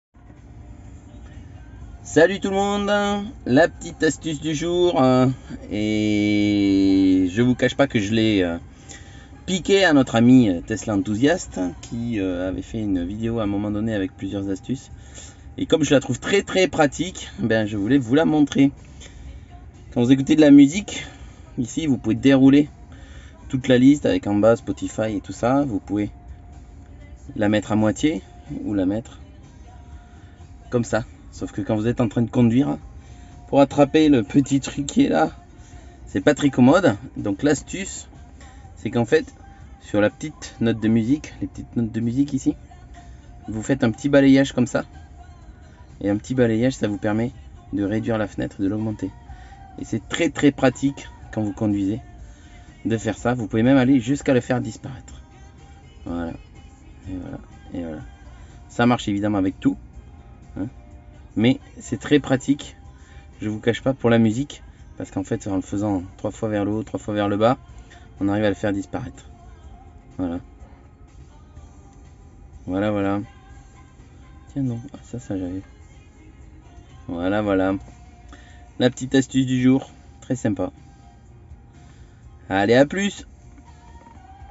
petite astuce du jour pour ouvrir les menus de musiques dans ma Tesla model 3 quand on conduit.
si vous souhaitez commander et bénéficier de 1500km de supercharge gratuite, n'hésitez pas à utiliser mon lien de parrainage